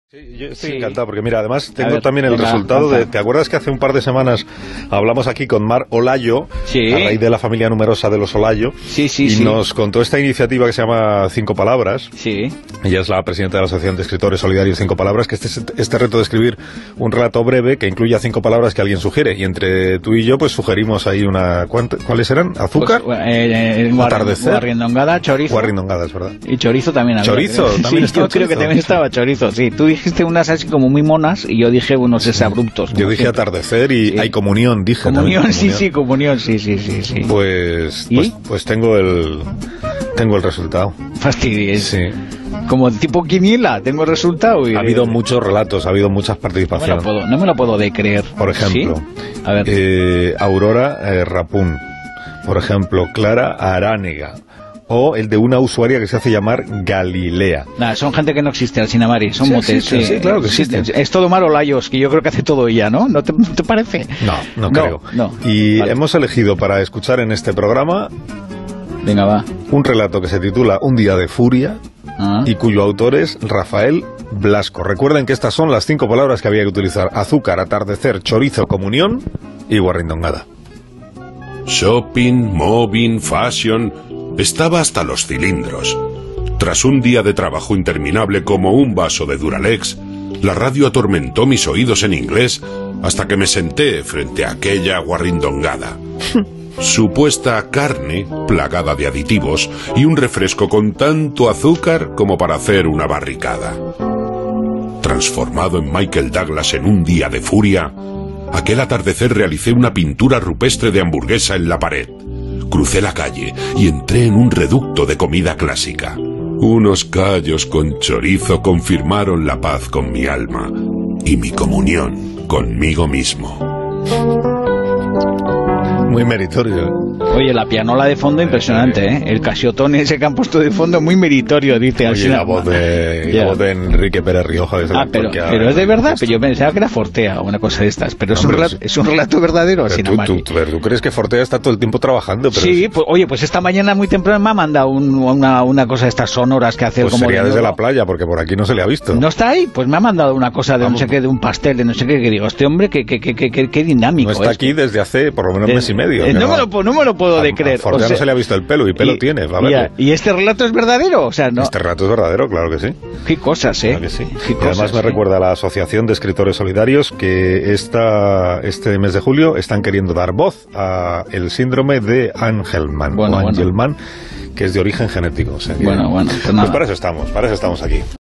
Carlos Alsina y Cinco Palabras. Relato de Rafael Blasco
Carlos Alsina recuerda las palabras que regalaron para nuestros relatos solidarios.

Hoy ha escogido para su programa:
 “UN DÍA DE FURIA”. Relato de Rafael Blasco con las Cinco Palabras de Carlos Alsina y David de Jorge, en la voz del actor de doblaje, Enrique Pérez Rioja.

En apoyo a nuestra causa de julio @asociacion_sindrome_d_angelman